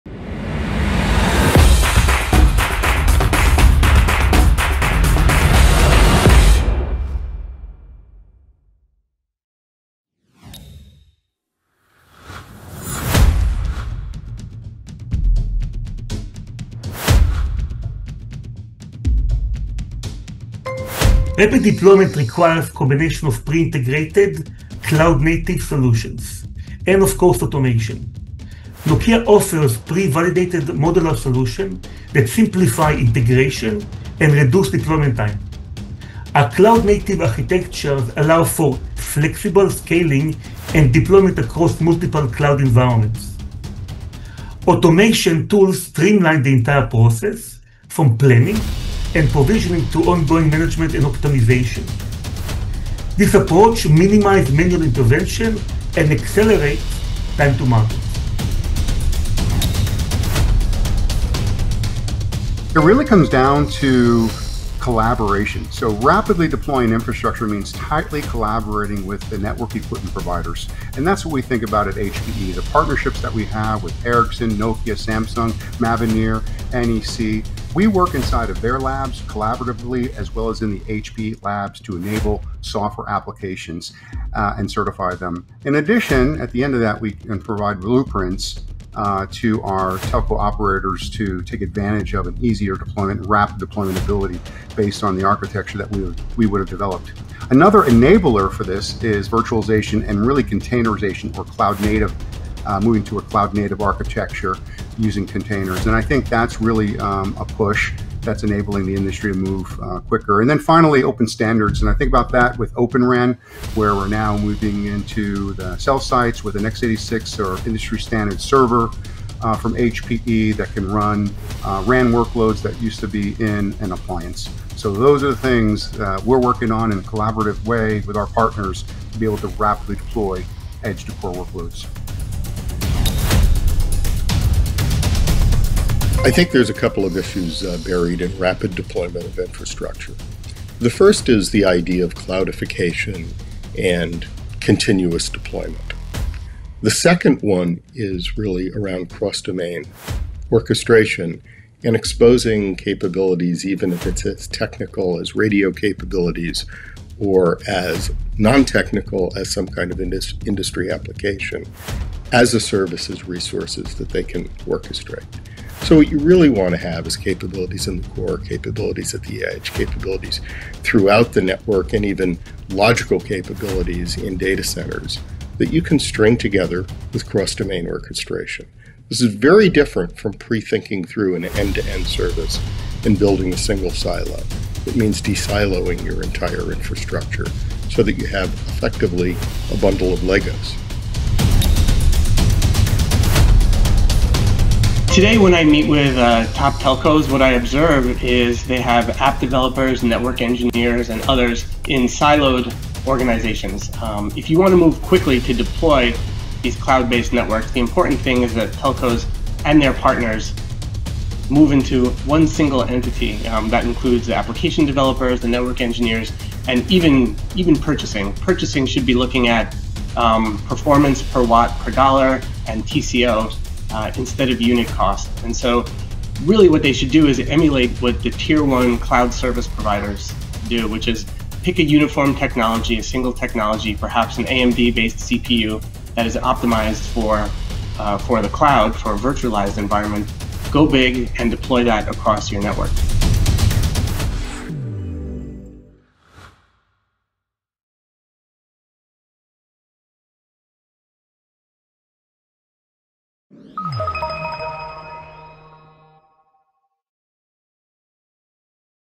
How can telcos rapidly deploy core-to-edge infrastructure?
In this informative session, experts from Nokia, HPE, Appledore Research and AMD delve into the critical aspects of rapidly deploying telecommunications infrastructure from the core to the edge. They discuss the significance of pre-integrated cloud-native solutions, automation and the crucial role of collaboration among network equipment providers. They discuss cloudification, continuous deployment and the benefits of adopting a unified technology strategy across the network. This discussion is essential for telecom professionals looking to understand the latest strategies and technologies driving faster and more efficient network deployments, and looks at the industry’s shift towards cloud-native architectures and the importance of cross-domain orchestration.

Featuring:
- Erez Sverdlov, VP, Cloud and Network Services, and Market Leader for Europe, Nokia
- Grant Lenahan, Partner and Principal Analyst, Appledore Research
- Phil Cutrone, SVP & GM, Service Providers, Telco, OEM, Hewlett Packard Enterprise
- Safy Fishov, Corporate Vice President of Data Centre OEM Sales, AMD